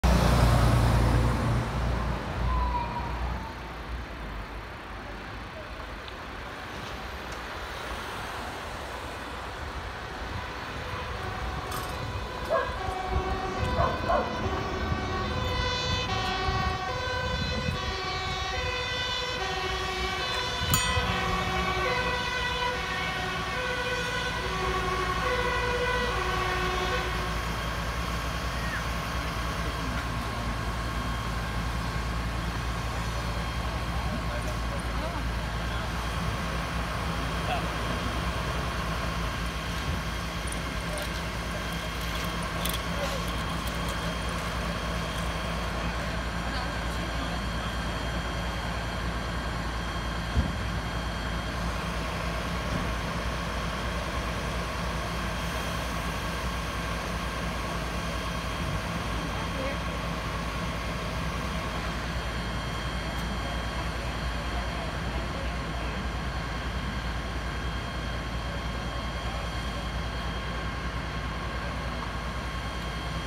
Woningbrand Insulindeweg 26.05.2022
Zojuist heeft de brandweer een ouder echtpaar uit een brandende woning gehaald. Oorzaak van de brand is nog onbekend.